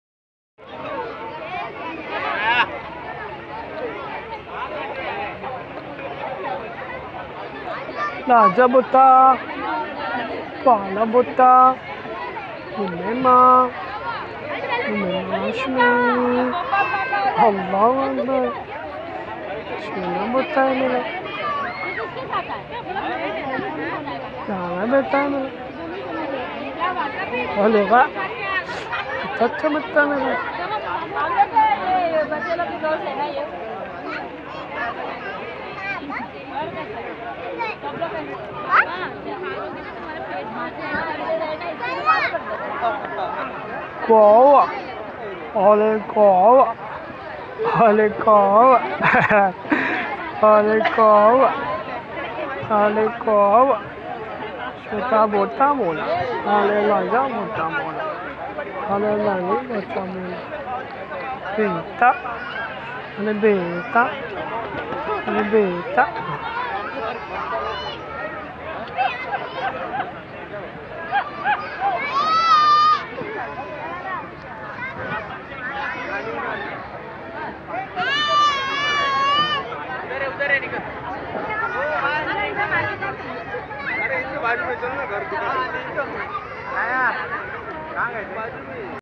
Little Umaima talking
On the occasion of marriage ceremony one of the frnd on 24 Dec 2018
Little Umaima talking and playing with her father.